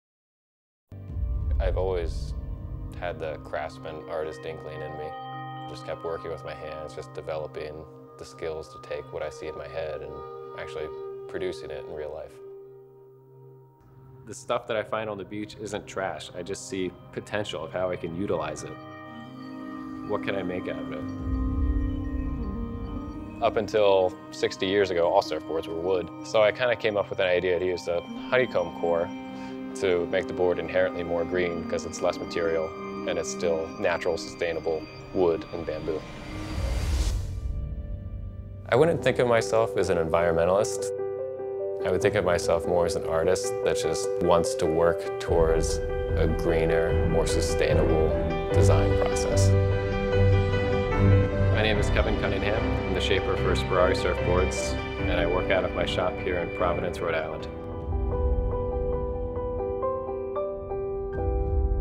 Kevin Cunningham - Eco Surfboard Maker
This amazing surfer made an Eco Surfboard!

Meet kevin cunningham.  a surf board shaper and craftsman who uses recycled materials to not only make boards but make a statement.  shot as part of the One Beach project for Barefoot wine.

Directed by Jason Baffa
Produced by Farm League